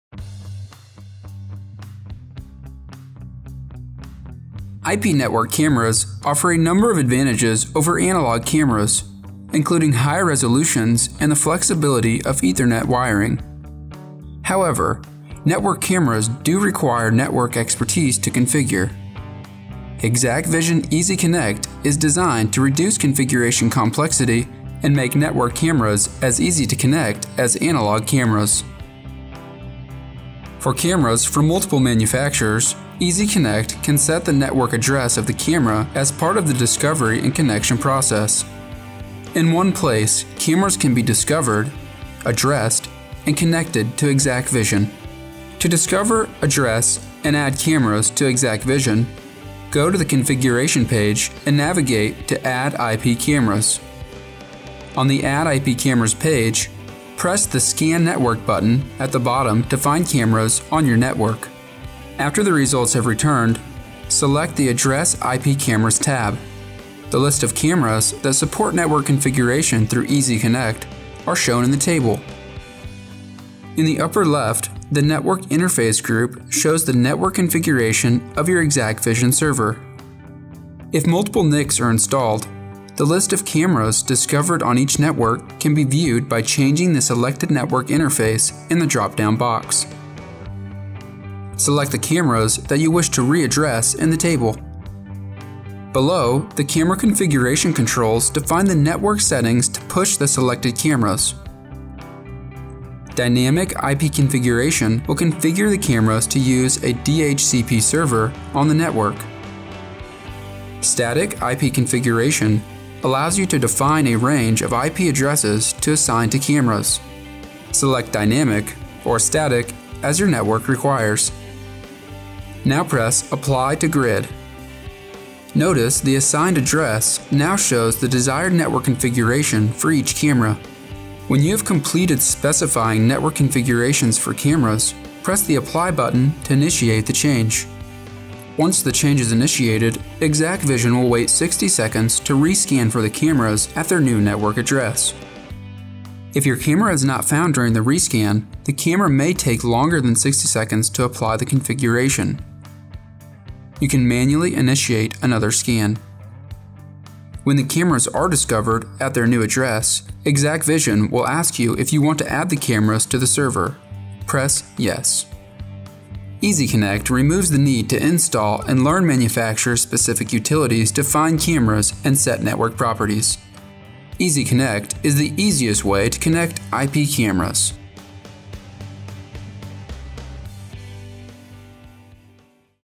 exacqVision, EasyConnect
The power of IP cameras with the simplicity of analog.

The new EasyConnect feature in exacqVision allows users to find, assign and address certain IP cameras to their network automatically. The exacqVision client scans the network to find new IP cameras and, with a few clicks, the selected cameras are assigned IP addresses and connected to a server.  This feature saves time and greatly simplifies the process of adding IP cameras to a modern day, network-based video surveillance solution.